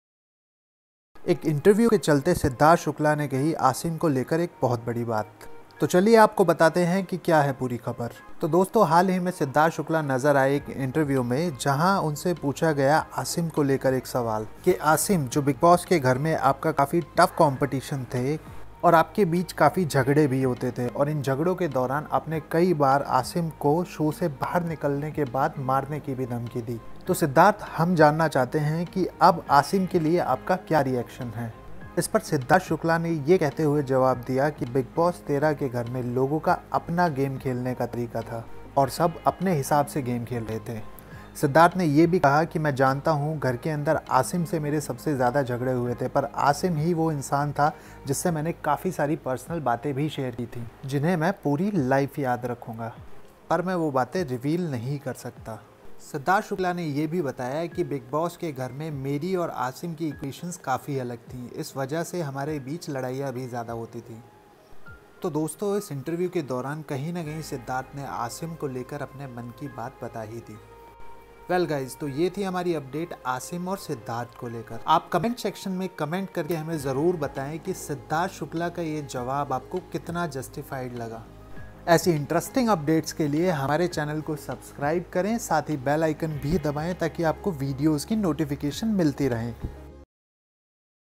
Siddharth Shukla Reaction: When Asked About Asim Riaz, Here's What He Said | Celebs Samachar
Siddharth Shukla comment on Asim Riaz during Interview.

Note: All the images/pictures shown in the video belong to the respected owners. I am no the owner of any pictures showed in the video.